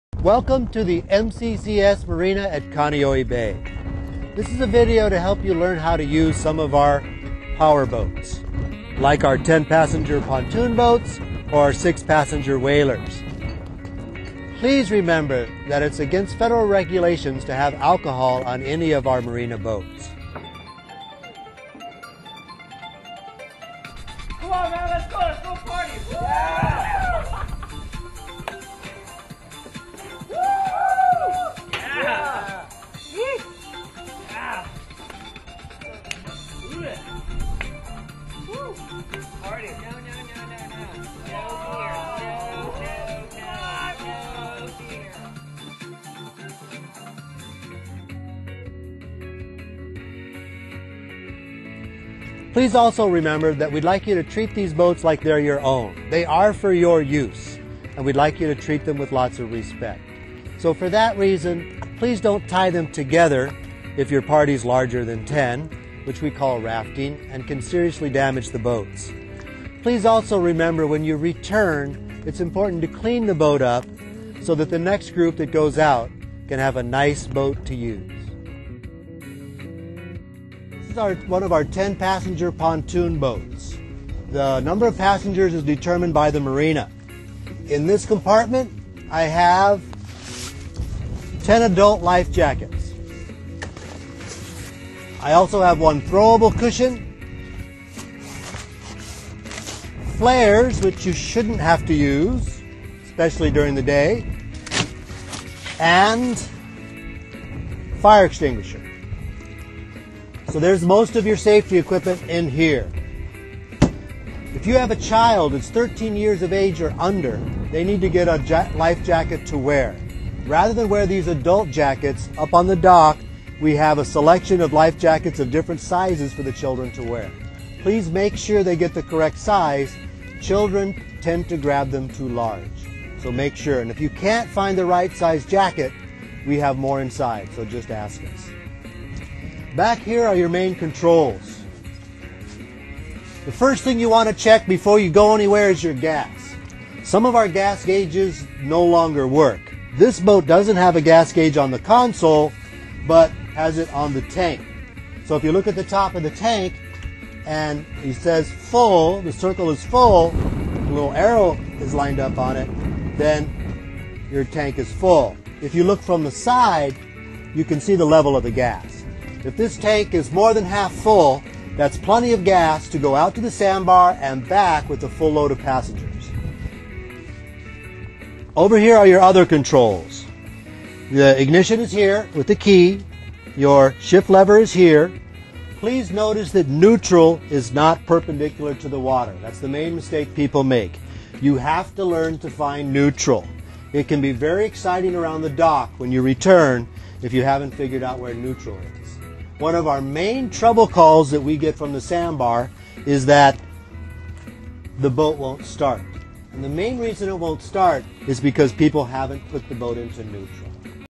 MCCS Marina Boat Video Part 1
This video is required watching for renting a powerboat at MCCS Hawaii Marina.